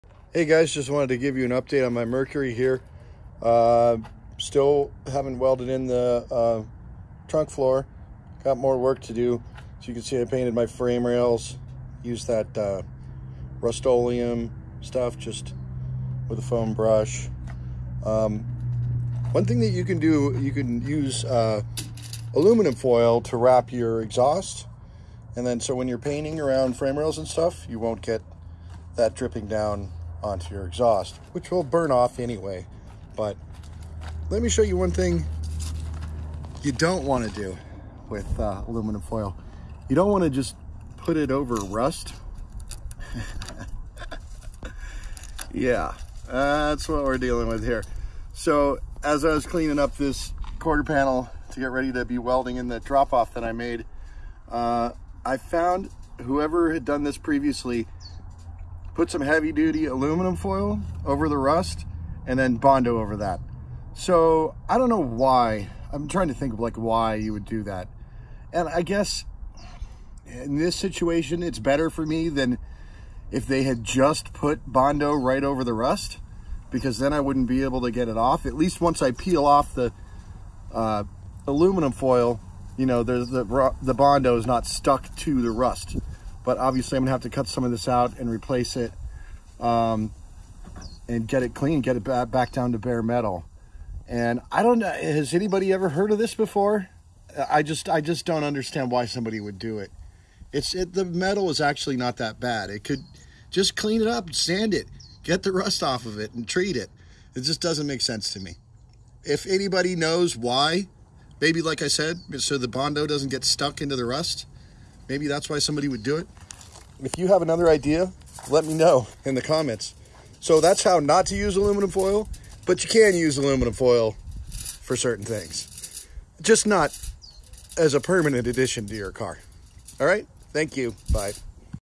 Using aluminum foil to fix rust?
The right way and the wrong way to use aluminum foil when restoring a car.

NOTE: If you have Bondo embedded into rust, you can burn it out with a torch.